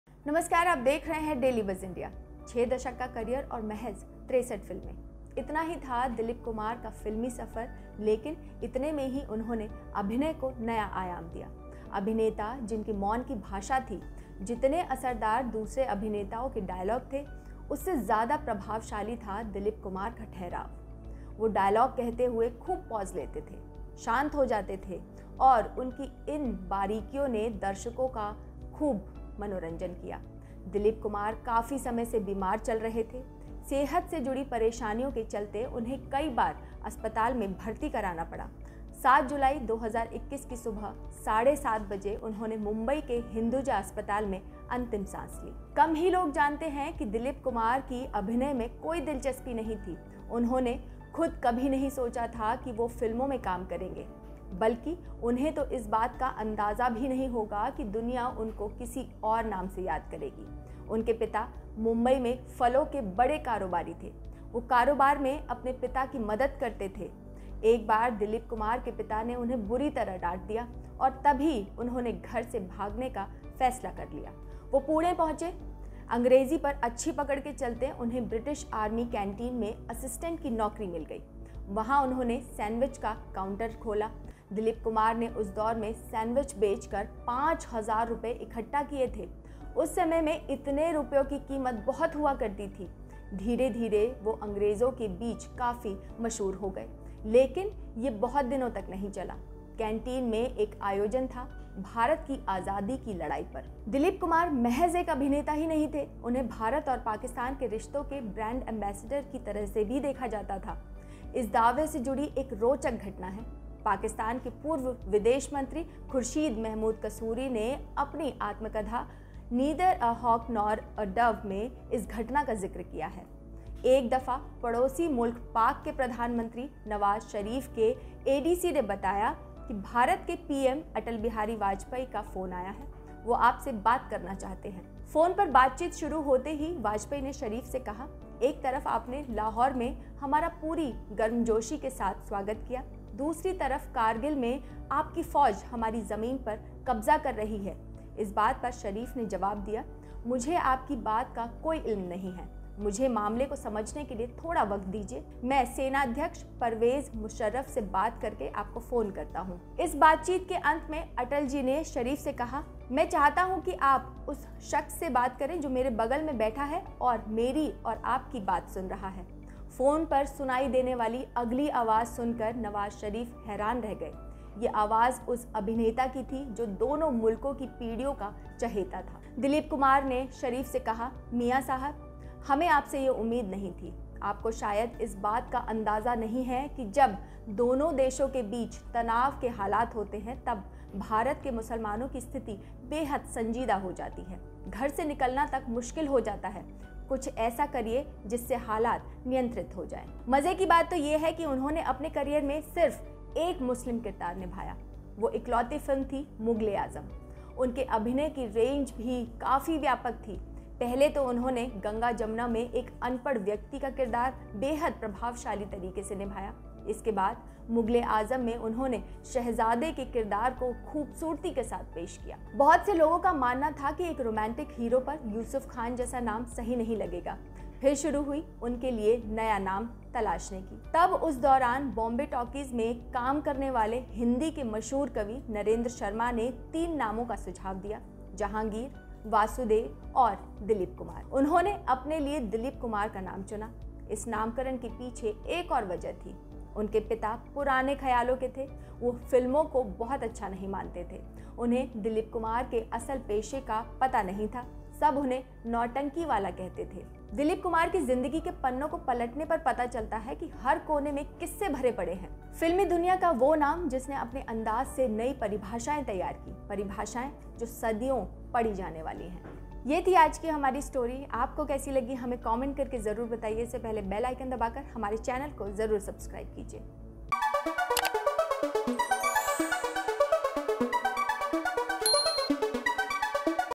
जब भारत के मुसलमानों के लिए Dilip Kumar ने लगाई थी Nawaz Sharif को फटकार
6 दशक का करियर और महज़ 63 फ़िल्में! इतना ही था दिलीप कुमार का फ़िल्मी सफर लेकिन इतने में ही उन्होंने अभिनय को नया आयाम दिया. अभिनेता जिसके मौन की भाषा थी. जितने असरदार दूसरे अभिनेताओं के डायलॉग थे उससे ज़्यादा प्रभावशाली था दिलीप कुमार का ठहराव. वह डायलॉग कहते हुए खूब पॉज़ लेते थे. शांत हो जाते थे और उनकी इस बारीकी ने दर्शकों ने के ज़ेहन में गहरी छाप छोड़ी. दिलीप कुमार काफी समय से बीमार चल रहे थे. सेहत से जुड़ी परेशानियों के चलते उन्हें कई बार अस्पताल में भर्ती कराना पड़ा